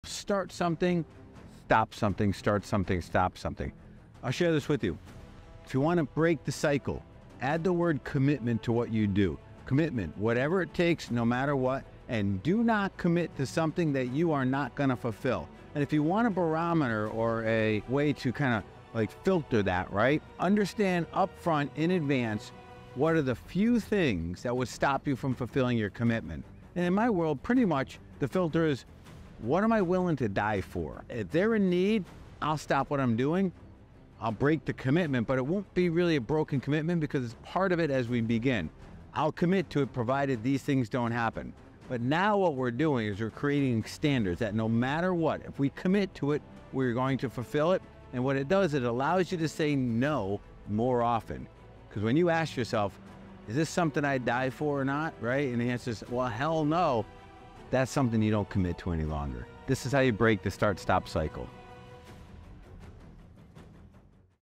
Commitment is Key
The Power of COMMITMENT 💪

We've all been there—starting something new with excitement only to find ourselves stopping and starting again. But I've learned the key to breaking this cycle lies in one word: COMMITMENT. When you truly commit to something, you add the power of determination, no matter the obstacles that come your way.

Here's the secret: Be clear about your commitments from the start. Understand what you're willing to go above and beyond for, and what you'd never give up on. For me, it's the things I'm willing to go to any lengths for, even if it means putting everything else on hold for a moment.

By setting high standards for what we commit to, we're empowering ourselves to say "no" to distractions that don't align with our true purpose. This way, we can focus on what really matters and follow through with unwavering dedication.

So, if you're ready to break the start-stop cycle, embrace the power of COMMITMENT. Hold yourself to the highest standards and let it guide your journey. No more hesitations—let's crush our goals and soar to new heights!